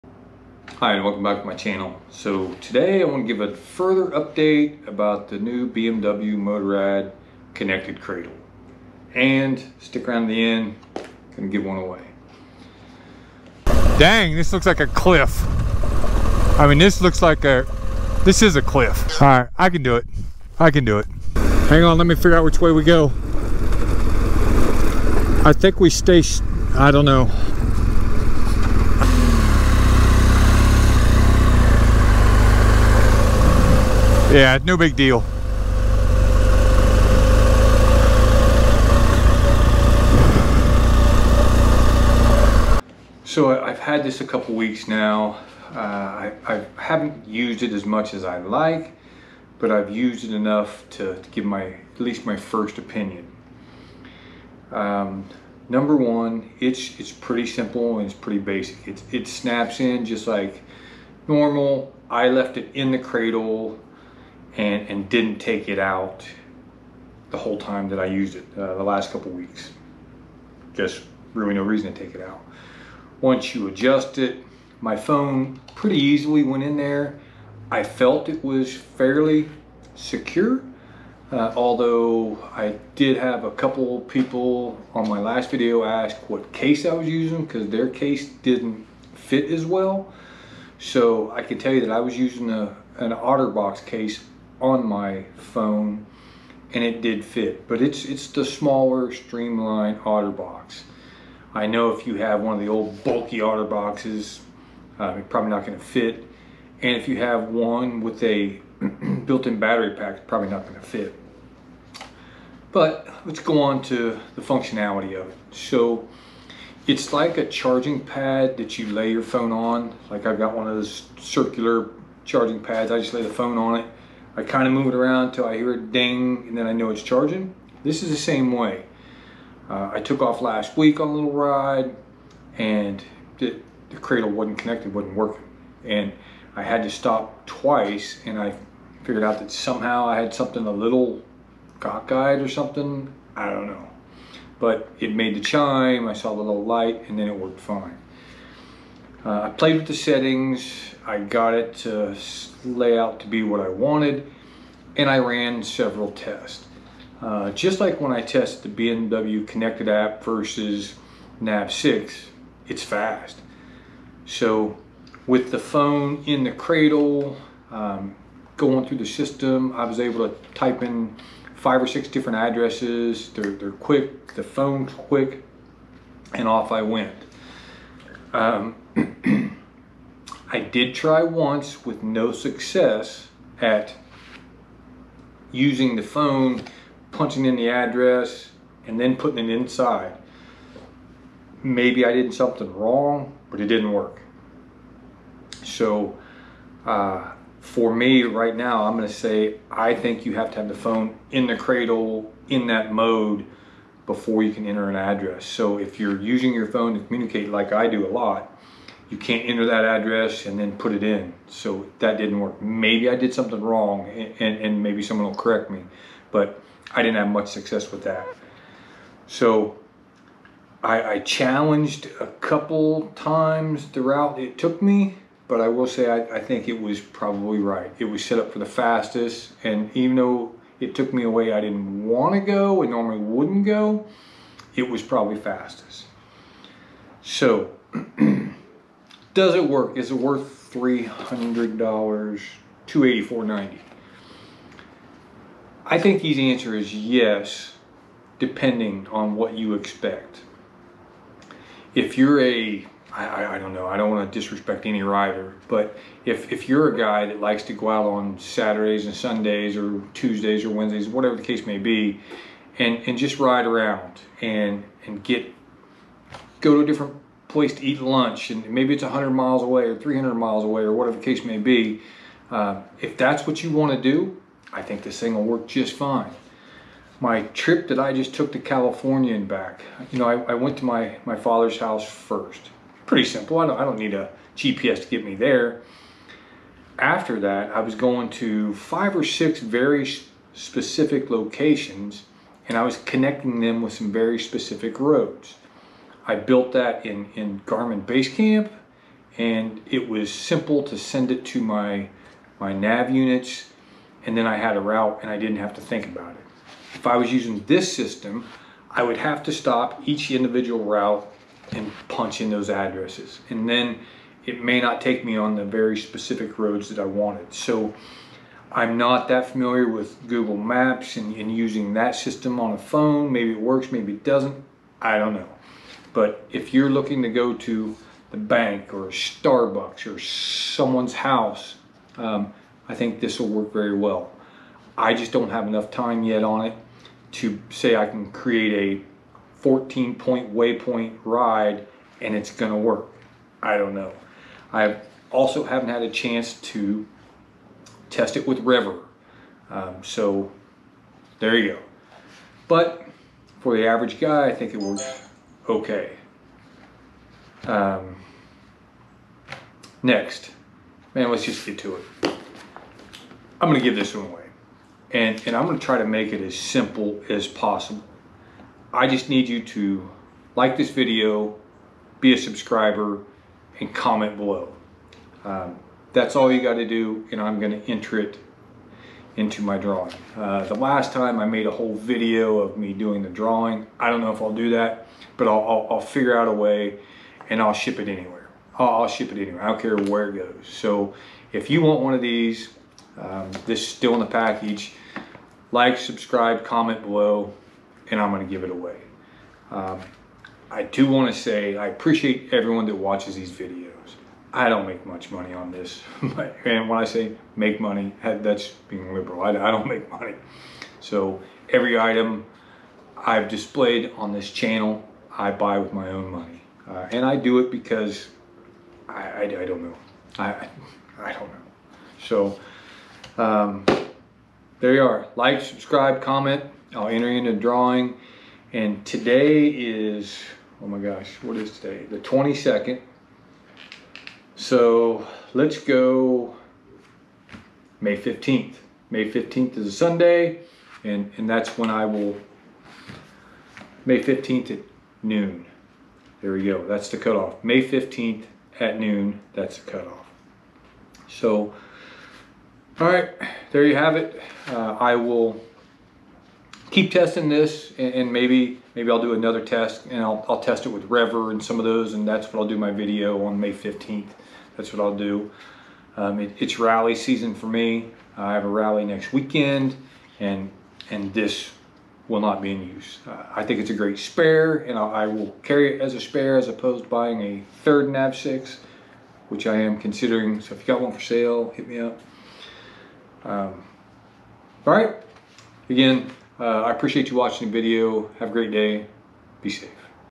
BMW Connected ride Cradle / opinions / Giveaway
I'm giving away a brand new never opened BMW Connected ride Cradle